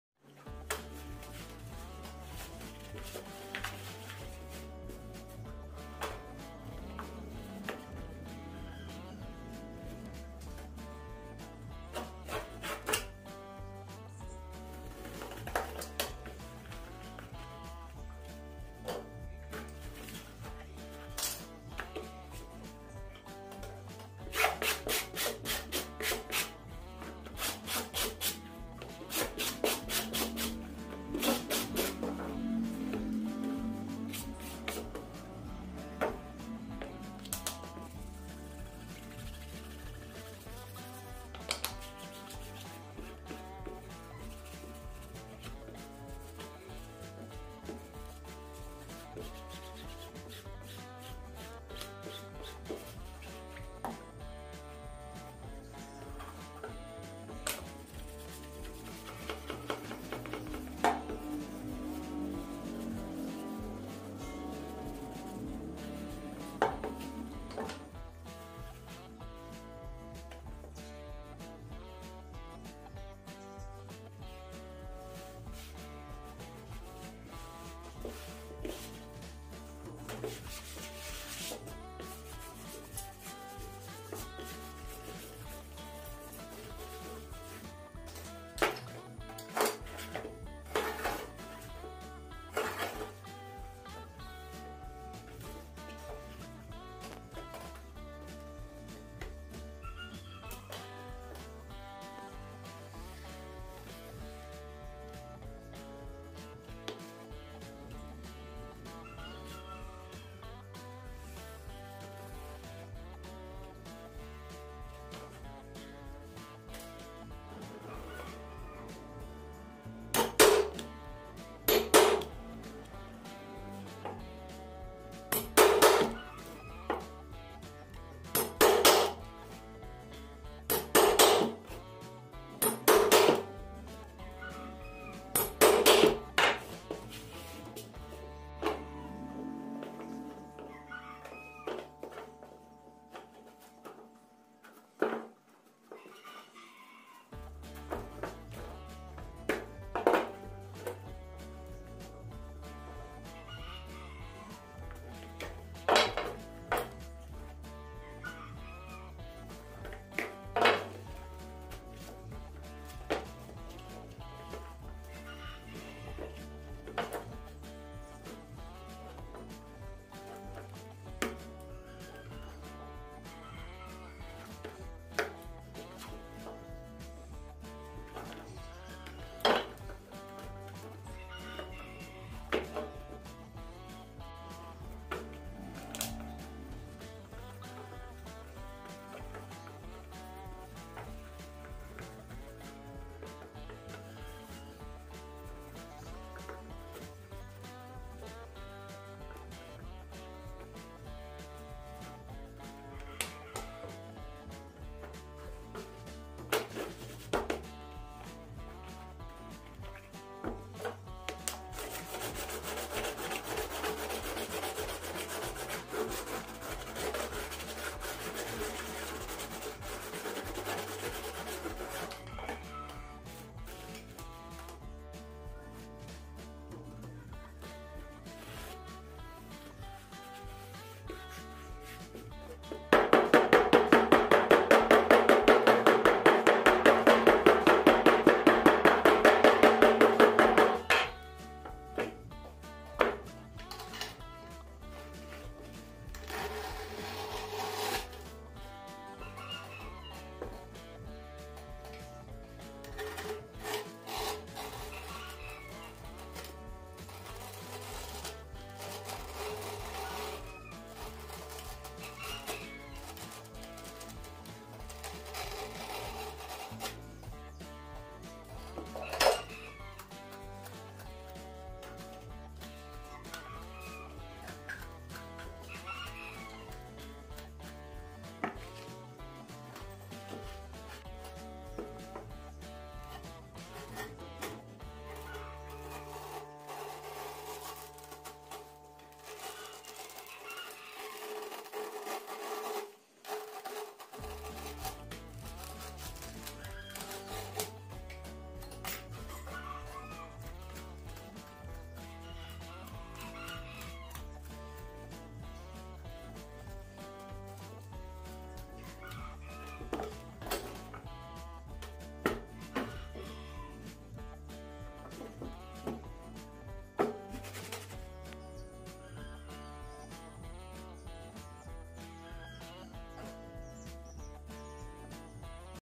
It's very easy to make
Making your own sandals is easy and fast, you can do it yourself at home.